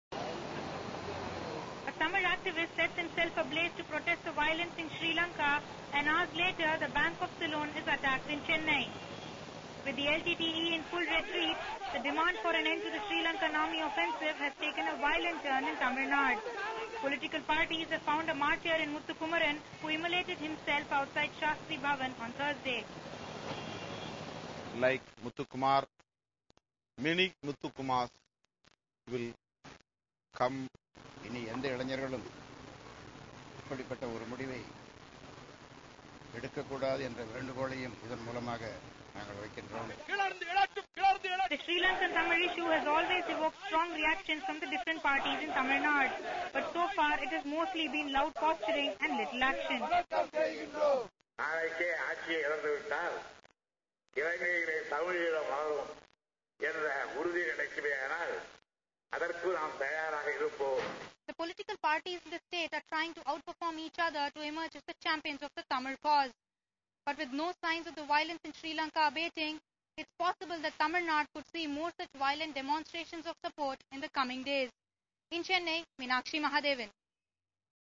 India welcomes Sri Lanka pause in war to free citizens
India has welcomed the Sri Lankan govt's announcement.